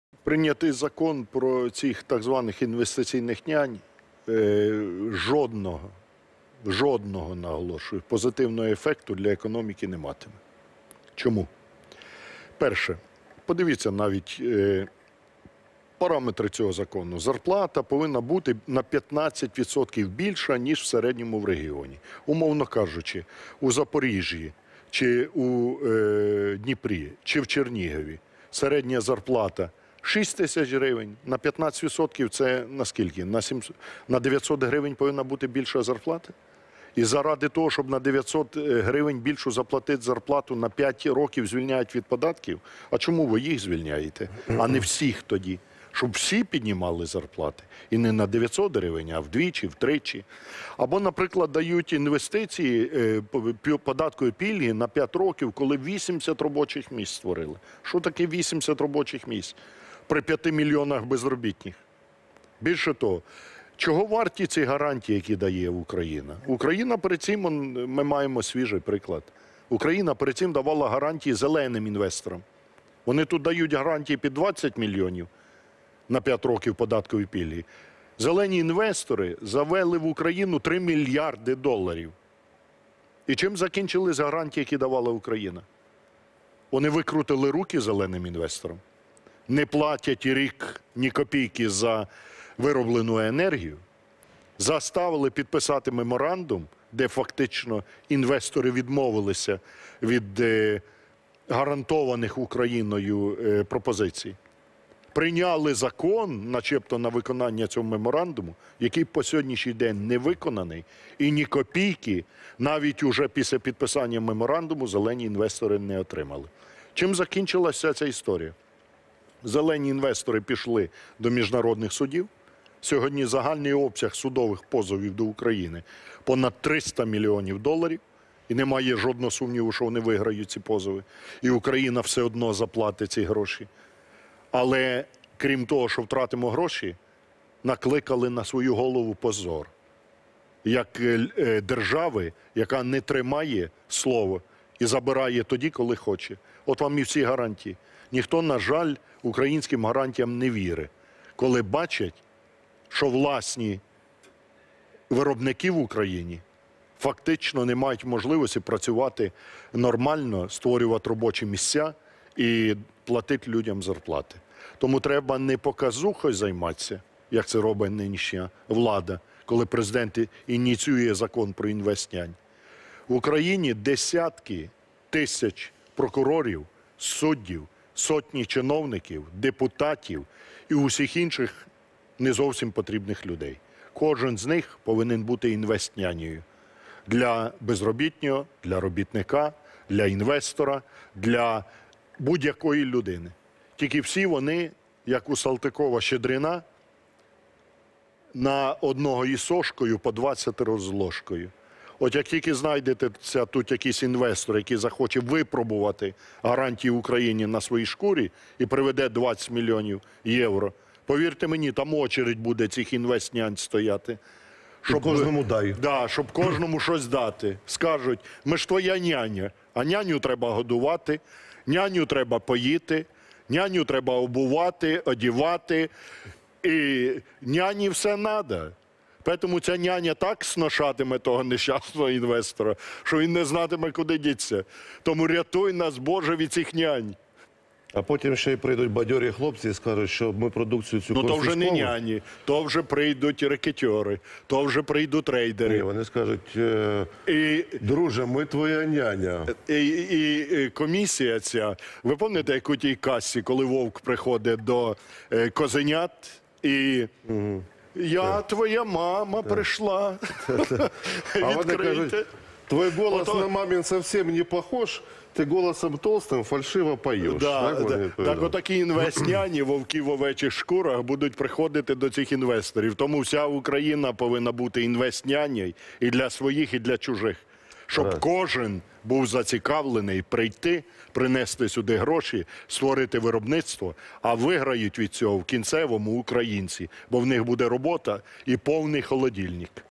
Ляшко про «інвестнянь»: Вовки в овечій шкурі
Прийнятий парламентом закон про так званих інвестиційних нянь жодного позитивного ефекту для економіки не матиме, переконаний Олег Ляшко. В ефірі телеканалу «112» лідер Радикальної партії обурився тому, що іноземних інвесторів влада ставить вище за своїх.
«За параметрами цього закону для того, щоб отримати податкові пільги, зарплата повинна бути на 15% більше, ніж середня в регіоні. Умовно кажучи, у Запоріжжі, Дніпрі чи Чернігові середня зарплата - 6 тисяч гривень, а за цим законом має бути на 900 гривень більше. І заради 900 гривень на 5 років звільняють від податків? Чому тоді не всіх звільняють, а тільки іноземців, щоб всі піднімали зарплати і не на 900 гривень, а вдвічі чи втричі? Або дають податкові пільги за створення 80 робочих місць. Що таке 80 робочих місць при 5 мільйонах безробітних в країні?», - обурився Ляшко.
Політик вважає, що не варті нічого і гарантії, які дає влада. Навів приклад «зелених» інвесторів, які привели в Україну 3 мільярди доларів.
«Де гарантії, які дали «зеленим» інвесторам? Влада викрутила їм руки, не платить рік жодної копійки за вироблену енергію, змусила підписати меморандум, де фактично інвестори відмовились від гарантованих Україною пропозицій, прийняла закон начебто на виконання цього меморандуму і до сьогодні він не виконаний. «Зелені» інвестори пішли до міжнародних судів. І загальний обсяг позовів до України становить понад 300 мільйонів доларів. Немає сумніву, що вони виграють ці позови. І в підсумку, крім того, що ми втратимо гроші, ми накликали позор як держави, яка не тримає своє слово», - сказав Ляшко.
Лідер Радикальної партії наголосив, що в Україні і так армія чиновників, які наглядають за бізнесом замість того, щоб допомагати.
«Як тільки знайдеться інвестор, який захоче випробувати гарантії України на своїй шкурі і приведе 20 мільйонів євро, повірте мені, там черга із цих «інвестнянь» стоятиме, щоб кожному щось дати. Казатимуть, як в тій казці про вовка і козенят: «Ми ж твоя няня». А няню треба годувати, поїти, обувати, одівати. Няні треба все. Тому та няня – вовк в овечій шкурі, так сношатиме того інвестора, що він не знатиме, куди дітися. Тому вся Україна повинна бути «інвестнянею» і для своїх, і для чужих», - підсумував Ляшко.